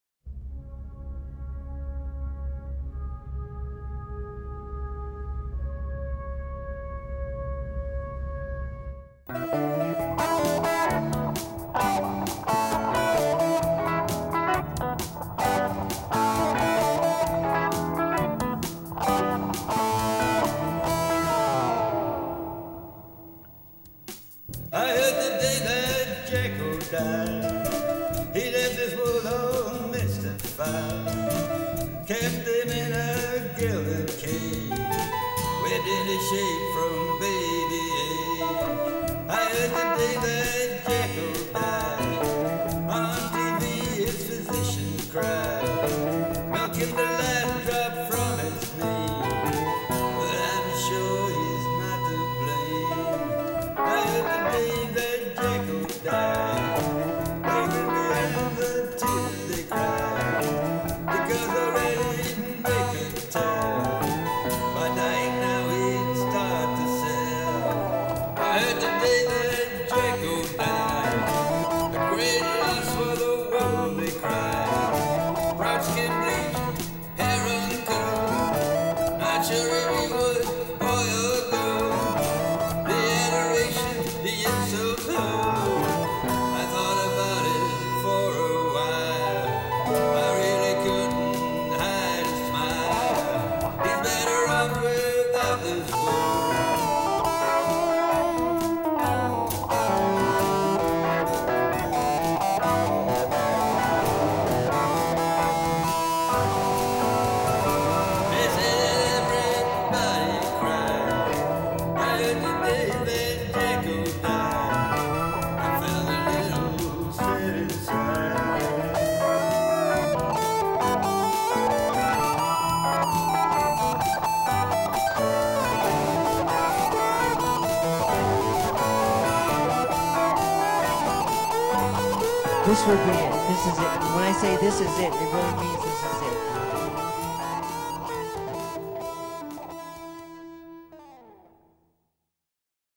Ode to Jacko
A little tribute to the sad 'King of Pop', deceased
I heard today that Jacko died
He left this world all mystified
Kept him in  a guilded cage
Whipped into shape from baby age

On tv his physician cried
Milking the last drop from his name
But I'm sure he's not to blame

They grinned behind the tears they cried
Because already they could tell
By dying now he'd start to sell

A great loss for the world, they cried
Proud skin bleached, hair uncurled
Not sure if he was boy or girl
The adoration, the insults hurled
I thought about it for a while
I really couldn't hide a smile
He's better off without this world

They say that everybody cried
I felt a little sad inside

This video is considered illegal in:
LOL